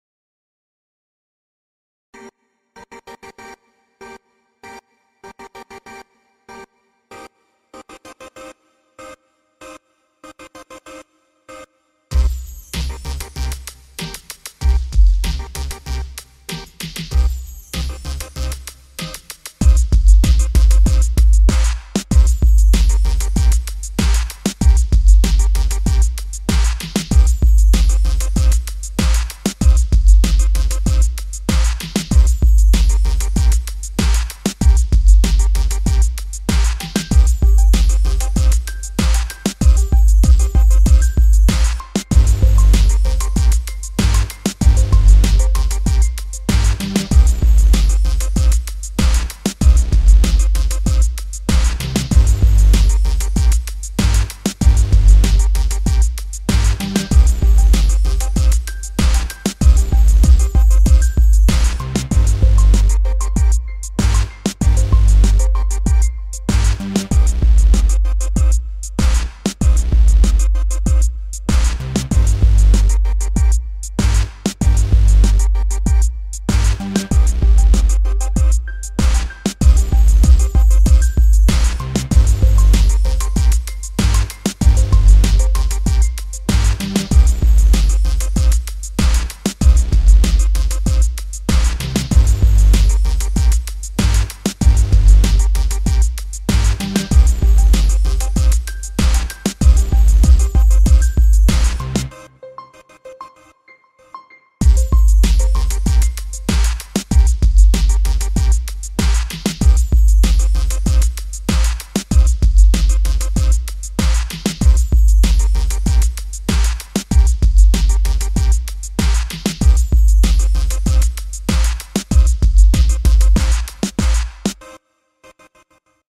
Hip Hop Beat Demo 1 by Skullbeatz
Unter dem Namen Skullbeatz produziere ich auch Hip Hop-Beatz.Wenn er euch gefällt,lasst ein Daumen hoch oder Kommentar.Wer ihn nutzen möchte,kann mir gerne schreiben.Dann kann ich den Kompletten Beat zuschicken.Bis denne.Haut rein.